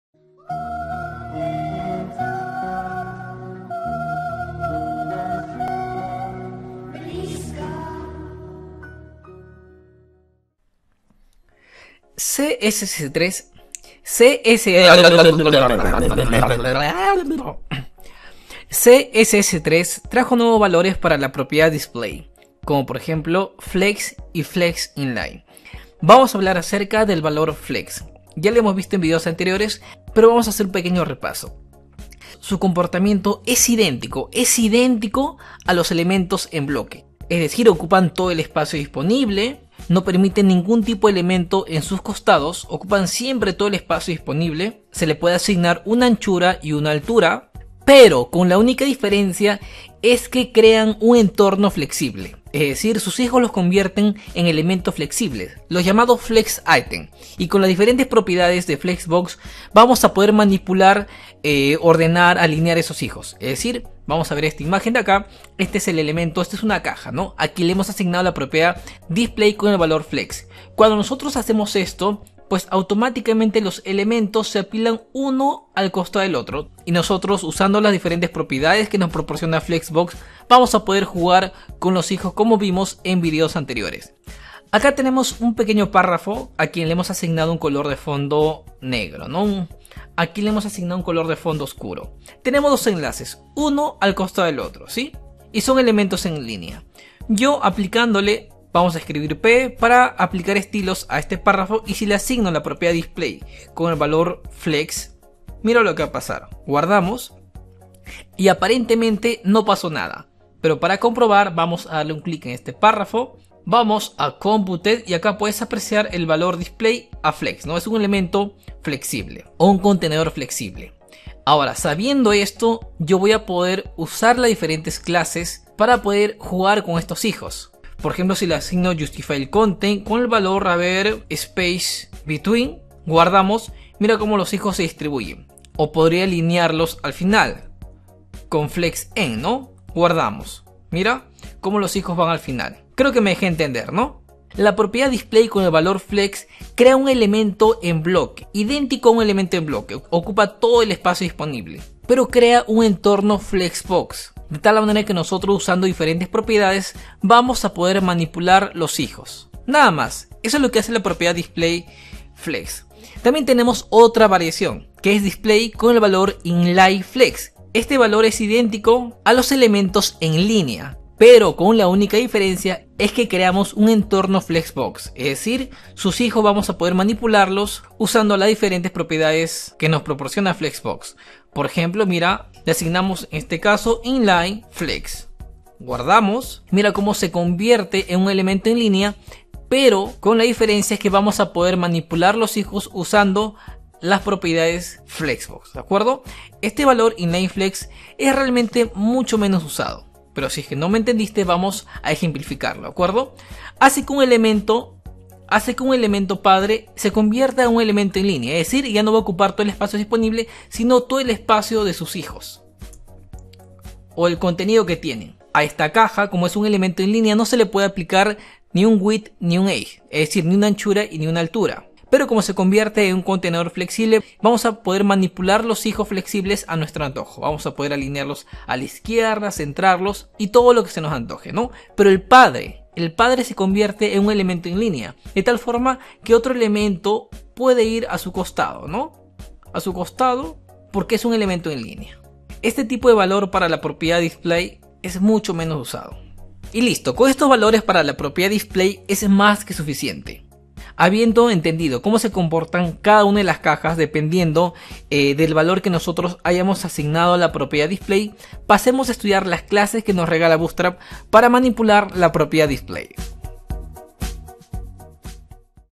Curso de Bootstrap 4 - #52 Display: "flex" e "inline-flex" en BOOTSTRAP 4 ✅
Bienvenidos al Curso de Bootstrap. En este video te hablaremos sobre "Display: flex" y "Display: inline-flex" . Dos valores para la propiedad display, muy importantes en el desarrollo web moderno.

Palabras claves (Ignorar):
- display flex  bootstrap
- curso de bootstrap
- curso de bootstrap 4
- cursos de bootstrap 4
- curso bootstrap
- tutorial de bootstrap